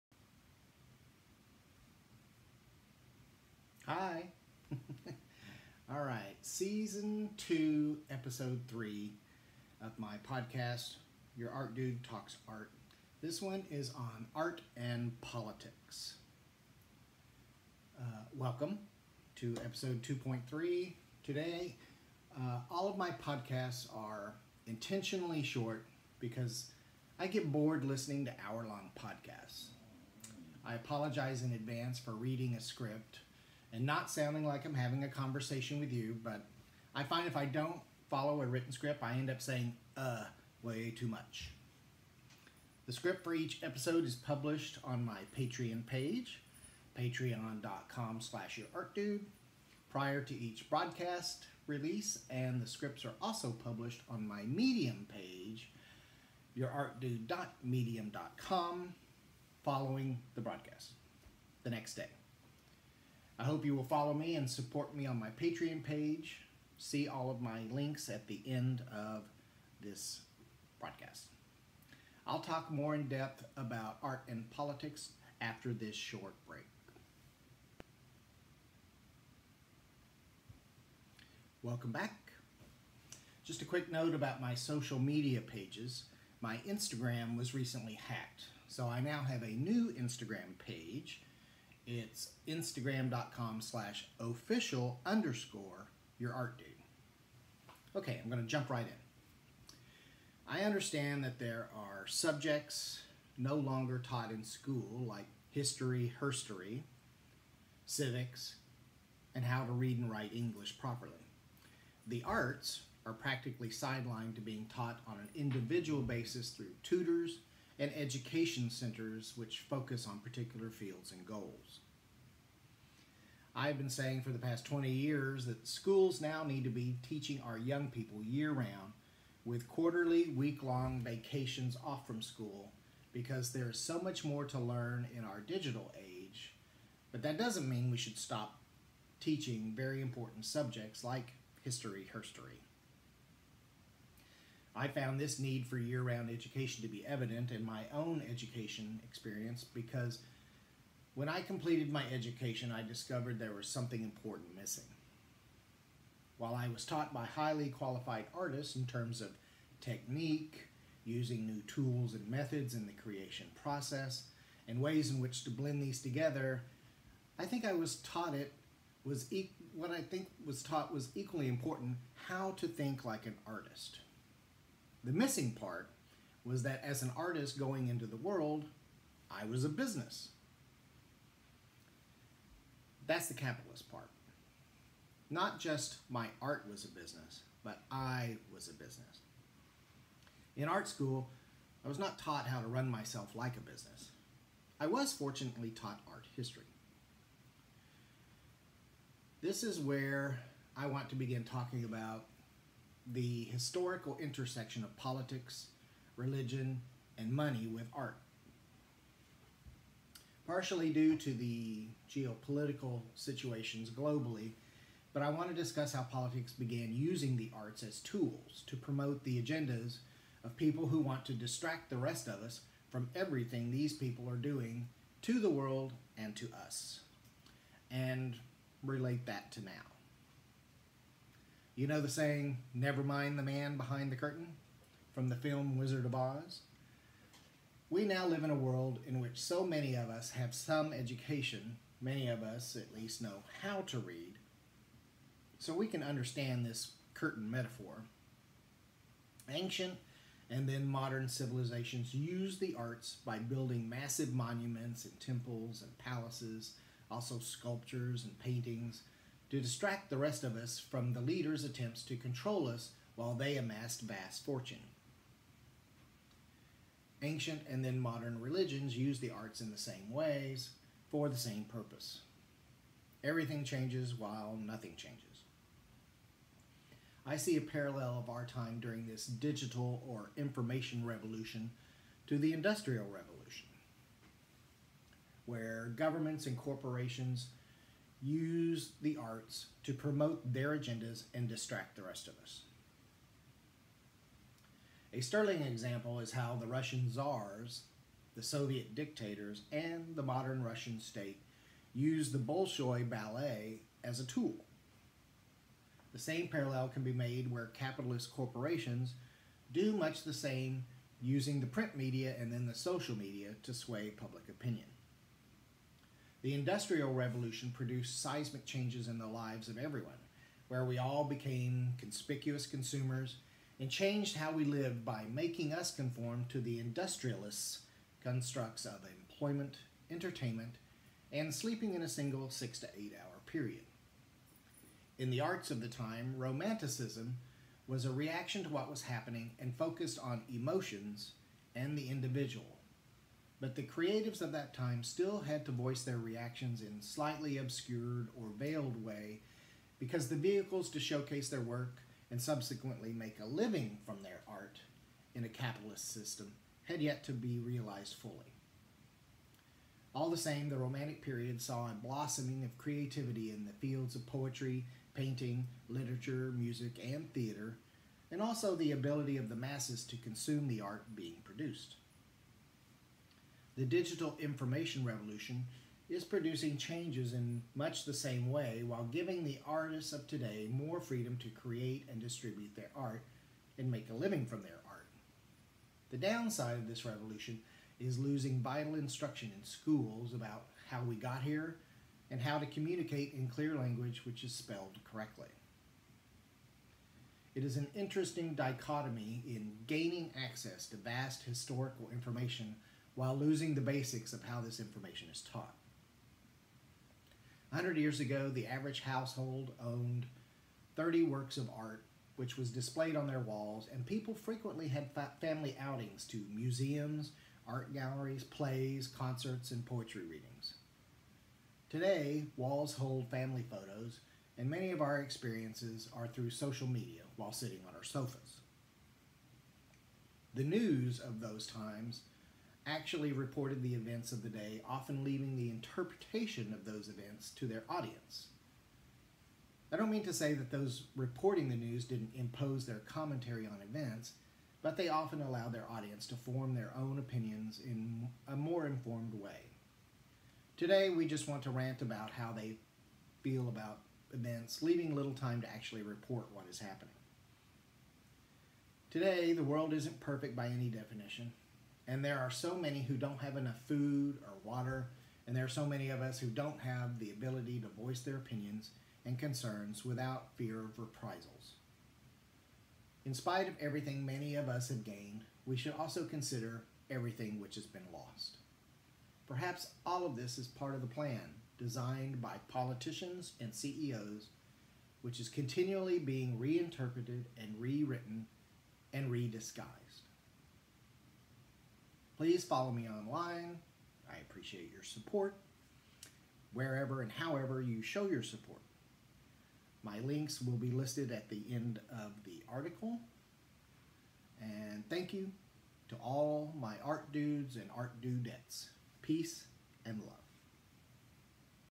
Art And Politics
Discussing the historical intersection of Art and Politics